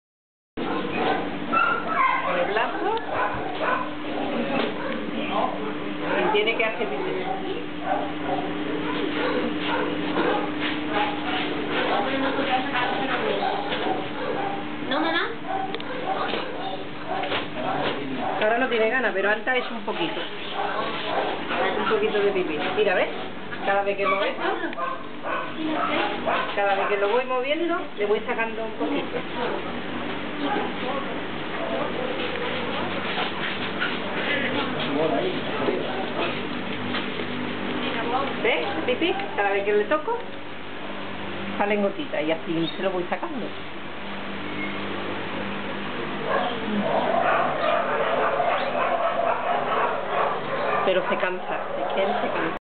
REY DESPUES DE SU SEGUNDA INTERVENCION...ARCA SEVILLA
REY, un perrito atropellado y dejado tirado en la carretera de donde se pudo rescatar. En pocos dias ha necesitado de dos grandes intervenciones en su columna, hay estamos con REY luchando por que pueda volver a andar y ser adoptado por una buena familia